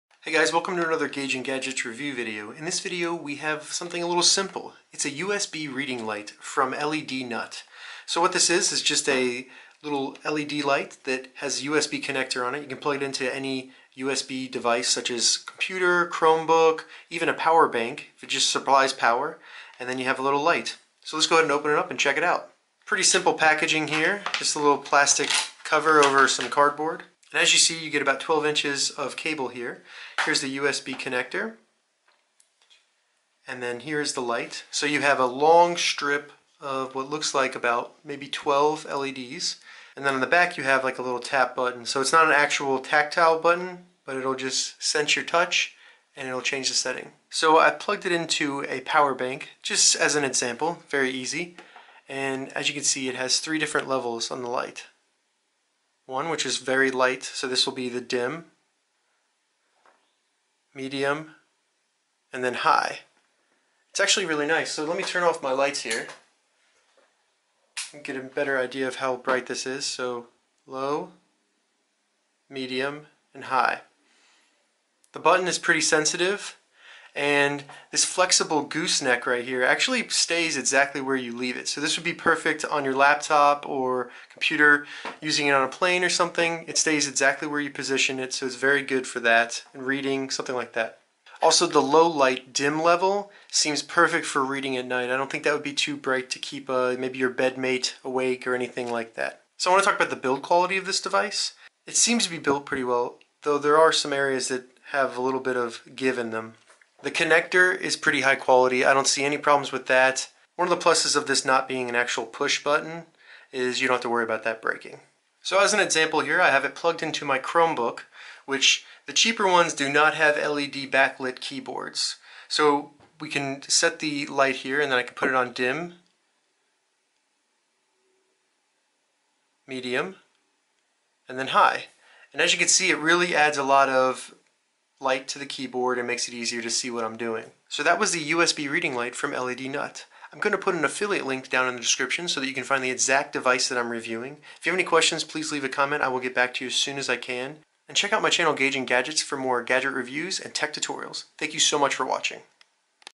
USB Reading Lamp Review - LEDNut Flexible Gooseneck USB Light
LEDNut USB Reading Lamp for Computer/Keyboard/Notebook/Laptop with Flexible Gooseneck, 3 brightness Settings and On/Off Switch
(paid link)

Looking for a flexible reading light? Check out the LEDNut USB reading lamp with extremely flexible gooseneck. This book light features a button with 3 differnt brightness settings and has over 10 LEDs

- Plug and play USB Lamp allows to work with Notebook, laptop, power bank, or any other powered USB connector.
- LEDNut USB keyboard light is your ideal work light at night.
- Stylish design matches any Macbook, Asus, Acer, Dell, PC
- Handy to illuminate the place the you need a light with the adjustable goose neck.
- The reading light is very light and is perfect for travel